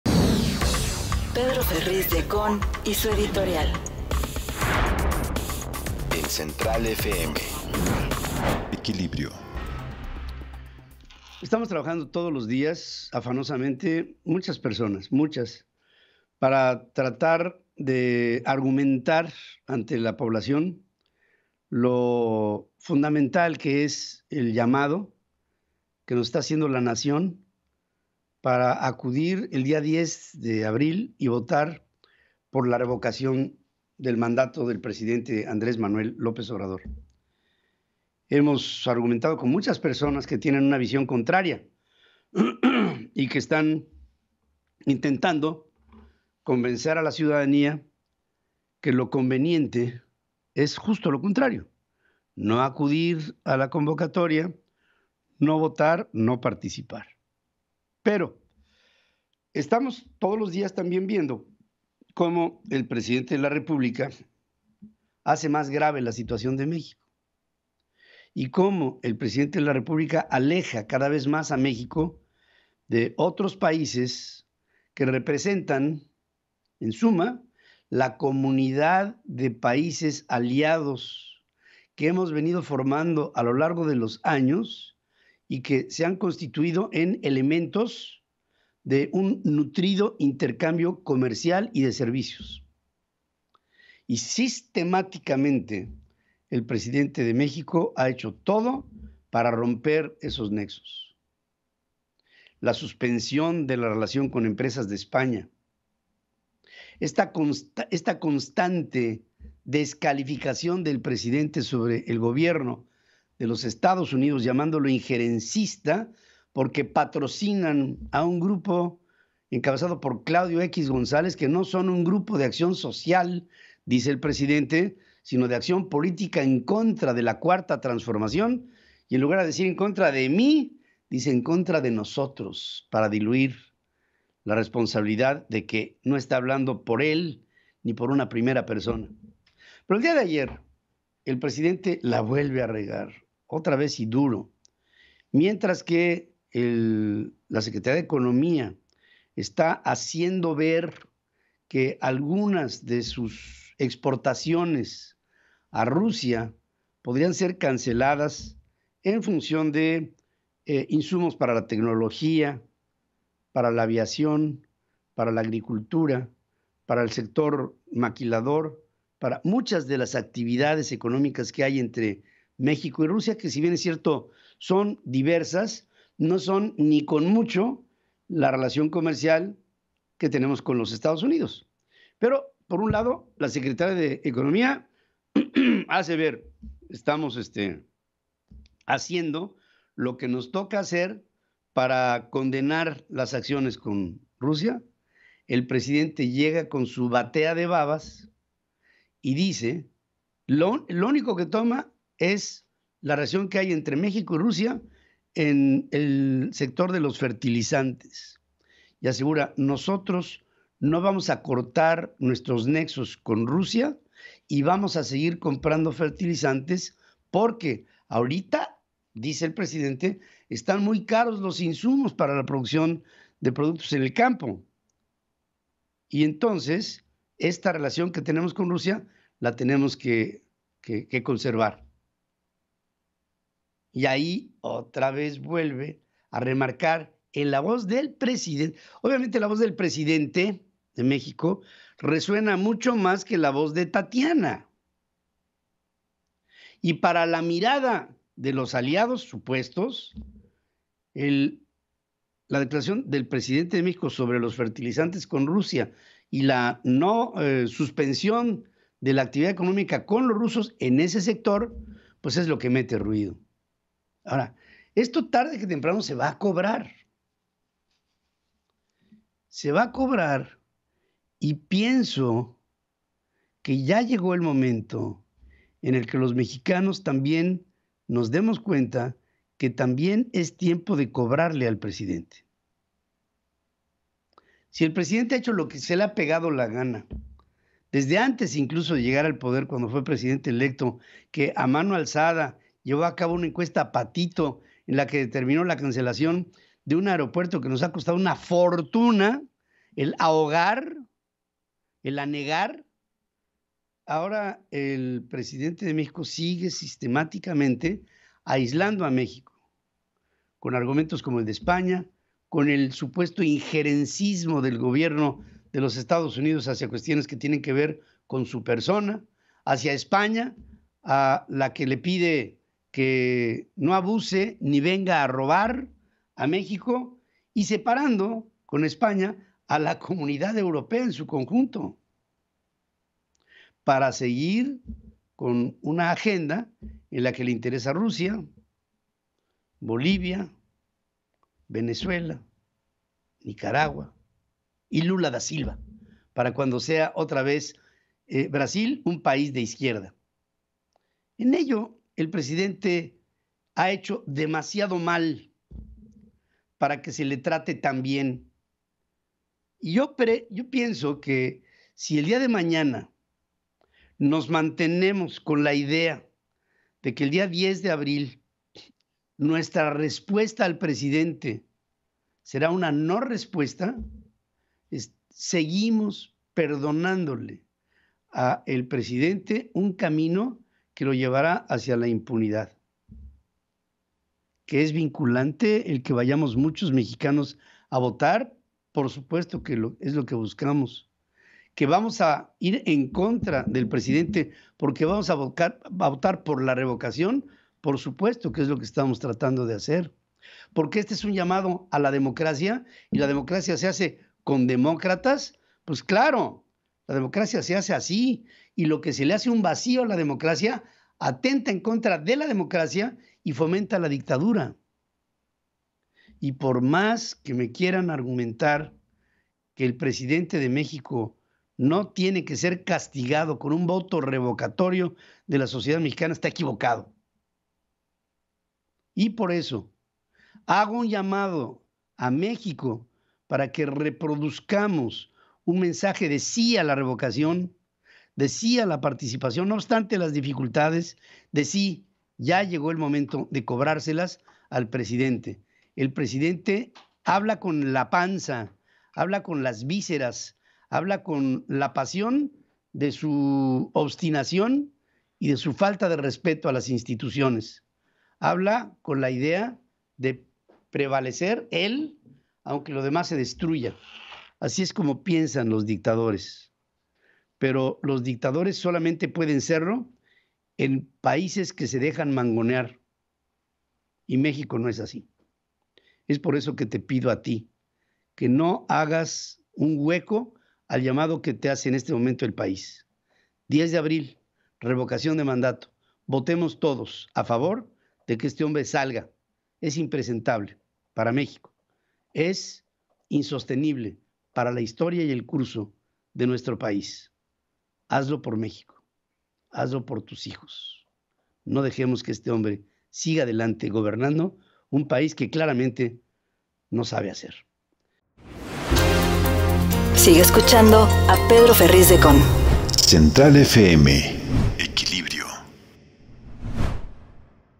AMLO aísla a México con declaraciones sobre Rusia, Ucrania, EU o España: Pedro Ferriz de Con
Pedro Ferriz de Con aseguró que todos los días, el presidente Andrés Manuel López Obrador “hace más grave la situación de México” y aleja al país de otras naciones que representan “la comunidad de países aliados que hemos venido formando a lo largo de los años”.